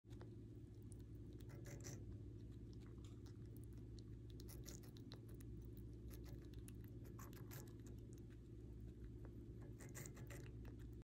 My hamster eating watermelon 🐹🍉
My hamster loves fresh food. I love watching him eat fruits. Watermelon is one of his favorite hamster fruits and it is safe to feed hamsters watermelon in moderation.  He eats it with so much passion and his little hamster paws are so cute holding it.